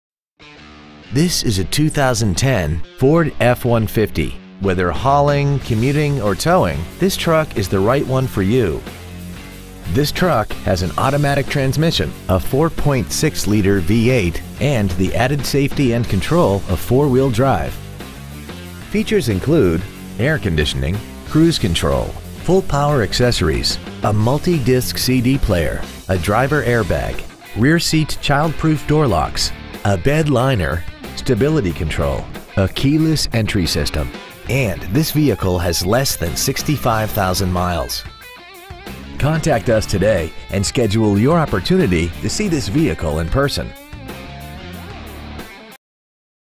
2010 Ford F-150 Columbus OH
Year : 2010
 Make : Ford
 Model : F-150
 Engine : 4.6L 8-Cylinder
 Trans . : automatic
 Exterior : Dark Blue Pearl Metallic
 Miles : 64,744
 Interior : Medium Stone

 2010 Ford F-150 Columbus OH